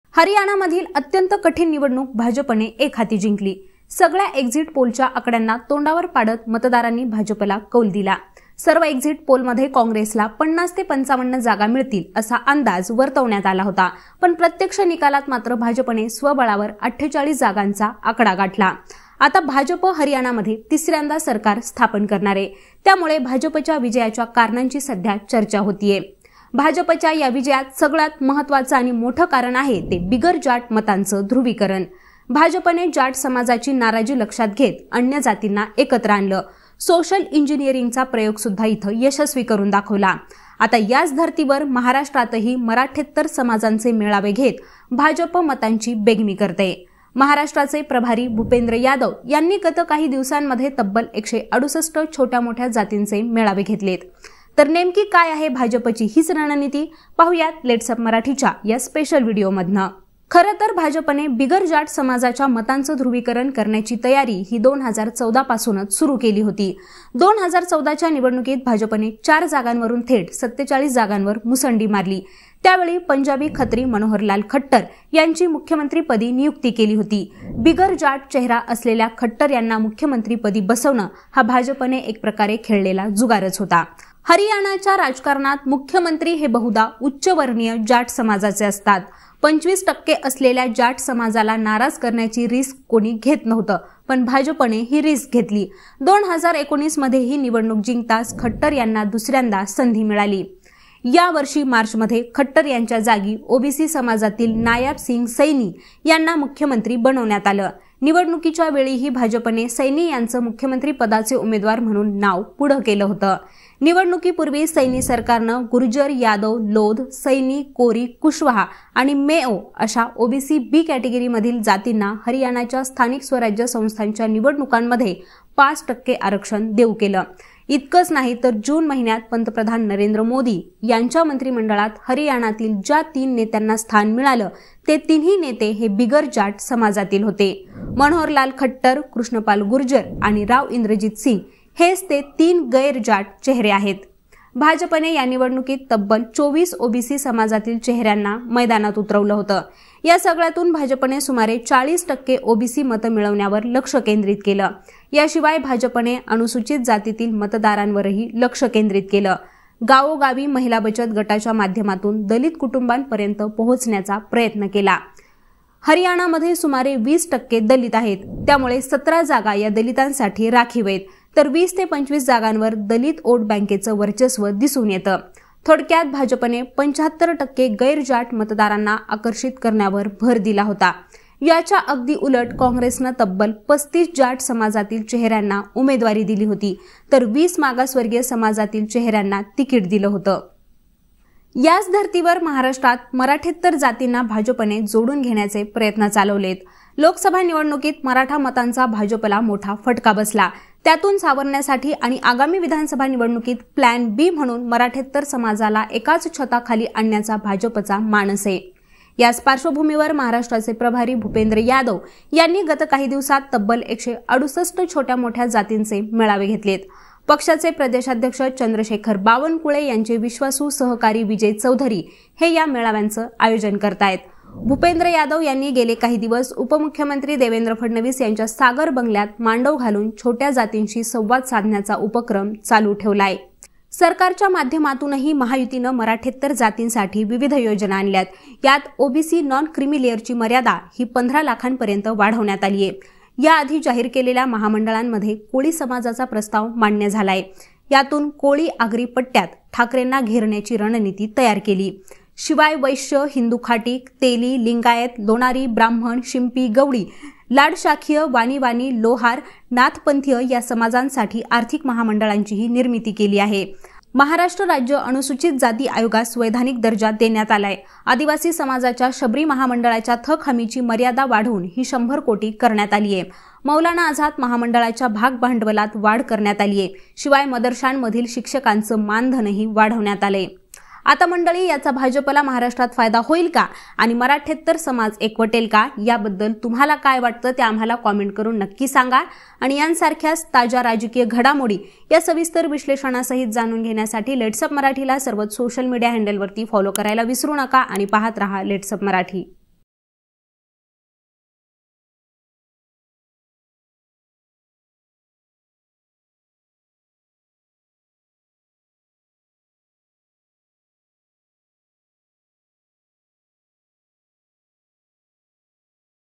Haryana Election : हरियाणाच्या धर्तीवर भाजपने महाराष्ट्रात मराठेतर मतांची मोळी बांधली | LetsUpp
हरियाणातील कठीण निवडणूक भाजपने एक हाती जिंकली. या विजयात बिगर जाट मतांचे ध्रुवीकरण हे सगळ्यात महत्त्वाचं आणि मोठं कारण ठरलं. याच धरतीवर महाराष्ट्रात मराठीत तर जातींना भाजपने जोडून घेण्याचे प्रयत्न चालवले आहेत. बिगर मराठा मतांची मोळी बांधून महायुती सरकारने मोठा डाव कसा आखला आहे? जाणून घ्या सविस्तर...